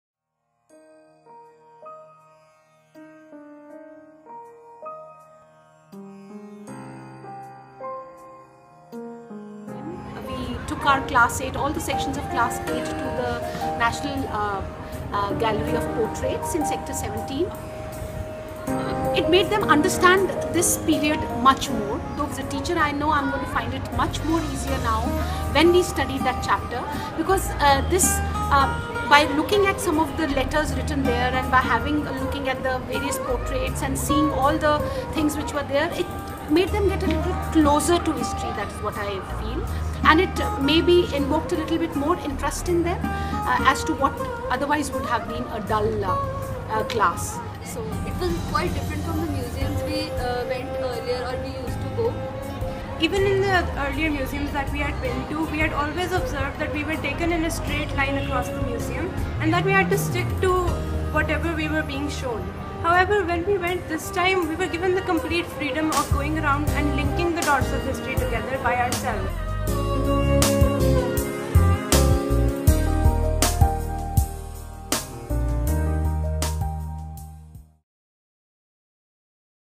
India's Struggle for Independence
A Museum Module for Schools using Creative Inquiry. Creative Inquiry allows students to look at history through the lens of other subjects such as English, Geography, Economics, etc and integrate their learnings.